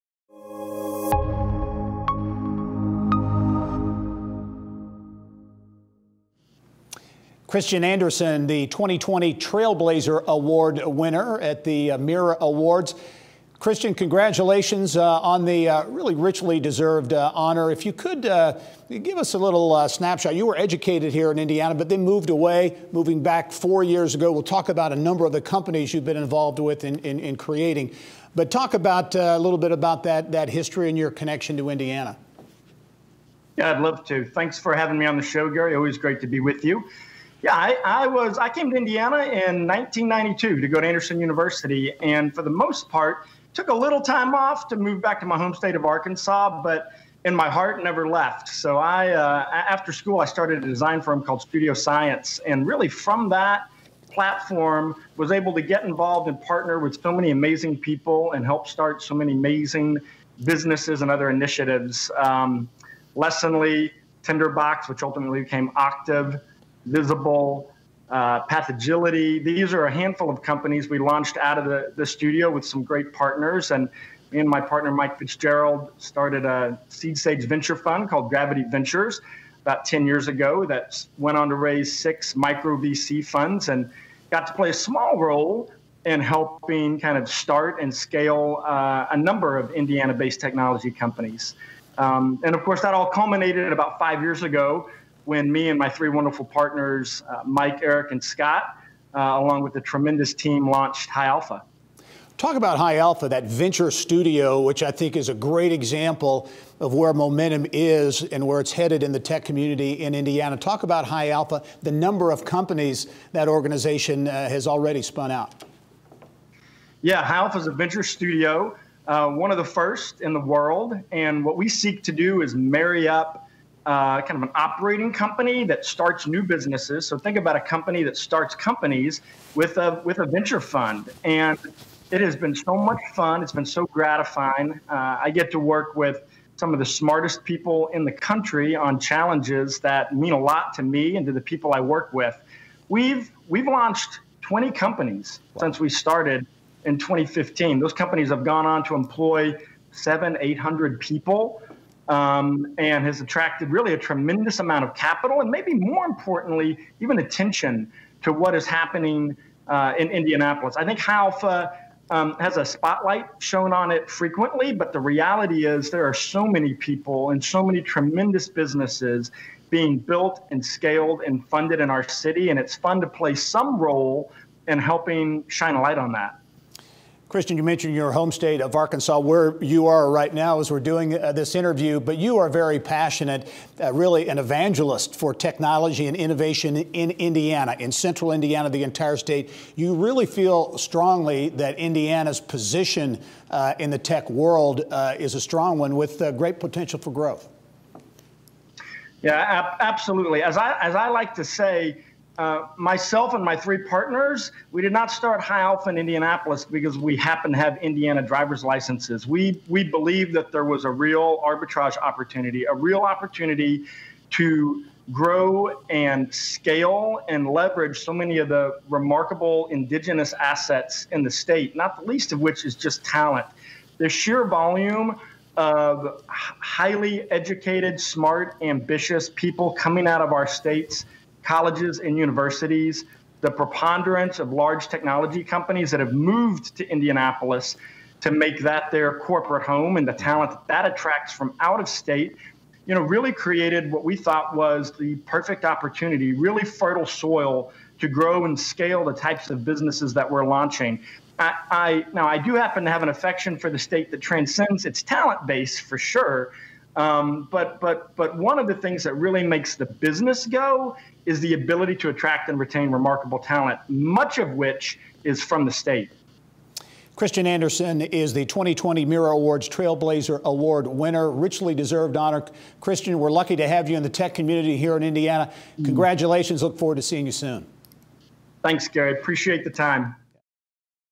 Kristian Andersen Receives TechPoint Trailblazer Award (2020)
Kristian Andersen, Partner, High Alpha – Trailblazer Award
Kristian Andersen is central to Indianapolis’ startup community and many prominent entities wouldn’t exist if not for him. He has vision to see the potential in people and organizations, a craftsmanship to distill and articulate that potential, a provocativeness that stirs people to act and think differently, and a boldness to take chances on new things or people.